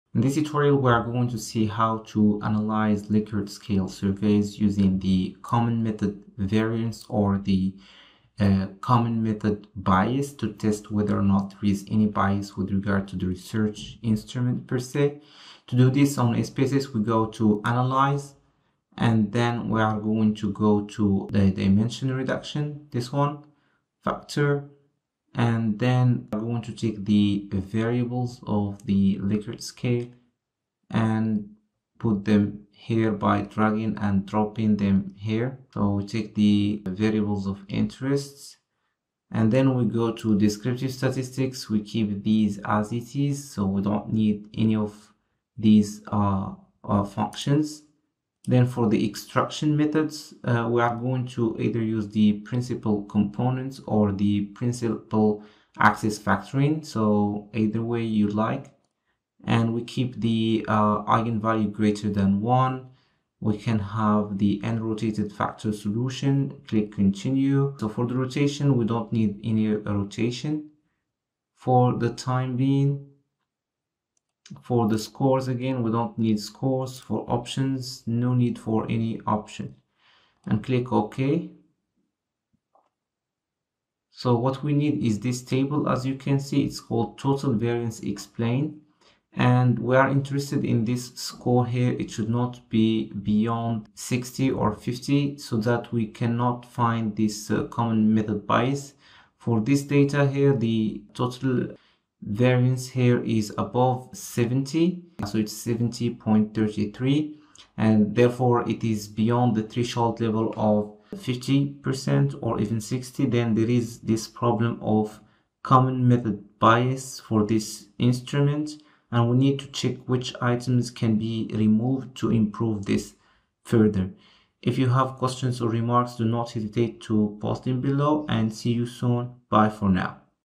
How to analyse Likert scale using Common Method Bias
Harman's single factor of Common method bias (CMV)
Steps to test Harman's single factor of Common method bias (CMV)
Step 1: Data Preparation
Make sure you have your dataset ready in SPSS with all the variables you want to assess for Common Method Bias.
Step 2: Factor Analysis
Go to "Analyze" in the top menu.
Choose "Dimension Reduction" and then select "Factor..." from the submenu.
Step 3: Select Variables
In the "Factor Analysis" dialog box, select all the variables you want to assess for Common Method Bias.
Move these variables to the right-hand side box, under "Variables."
Step 4: Options
Click on the "Extraction" button.
In the "Factor Extraction" dialog, choose the "Principal Component Analysis" method.
Make sure you check the box that says "Scree plot."
Step 5: Run the Analysis
Click the "OK" button to run the analysis.
Step 6: Examine the Scree Plot
After running the analysis, you'll get a scree plot that displays the eigenvalues for each factor. Common Method Bias suggests that if there's a single factor accounting for the majority of the variance, it may indicate the presence of CMB.
Step 7: Interpret the Results
Look at the scree plot. If you see a single dominant factor that explains a large proportion of the variance (usually more than 50%), it might suggest the presence of common method bias.